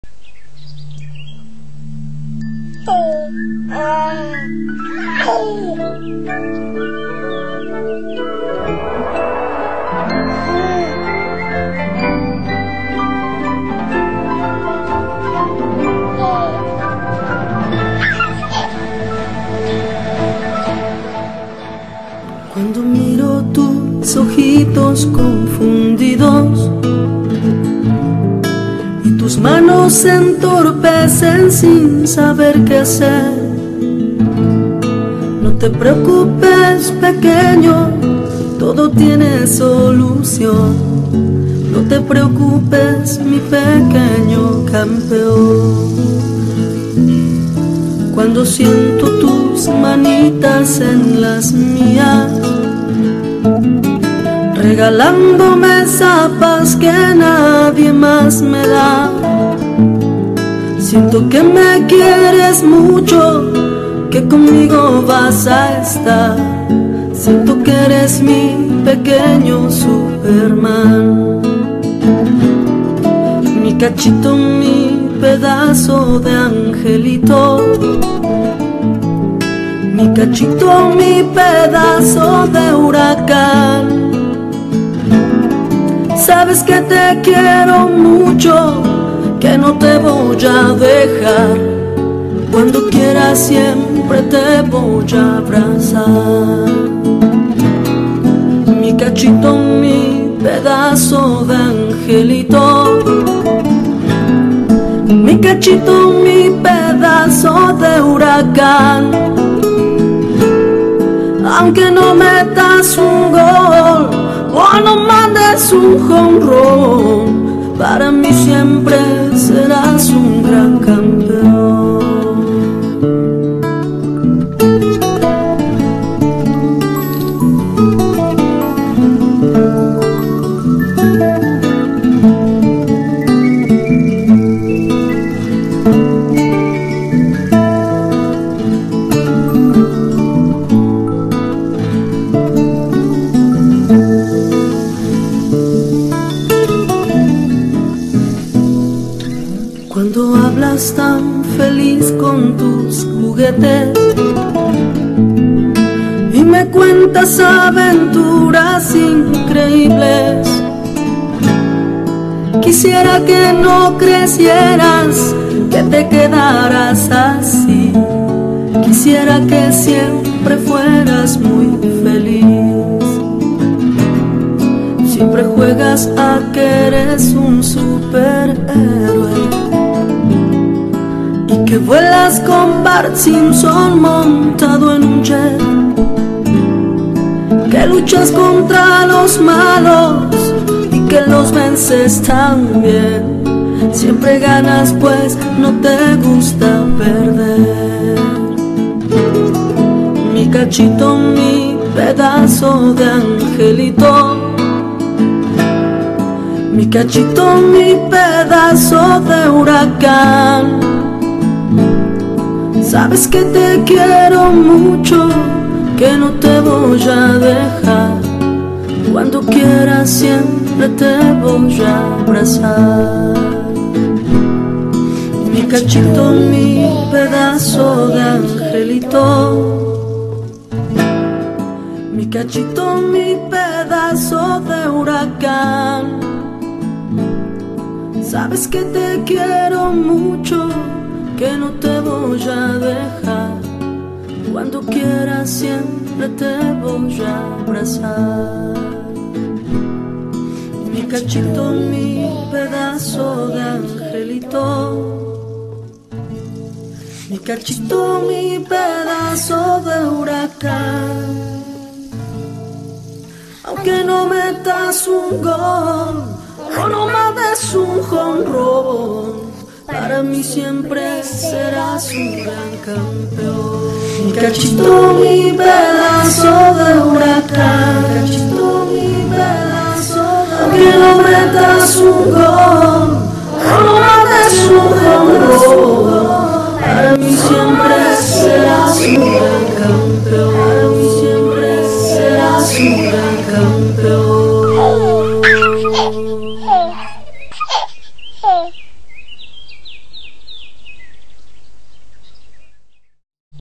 Mi Cachito NICKY ALEXANDER
Un regalo para una madre estupenda.. Felicitaciones por tu nenito... para que lo disfruten sus abuelos y amigos.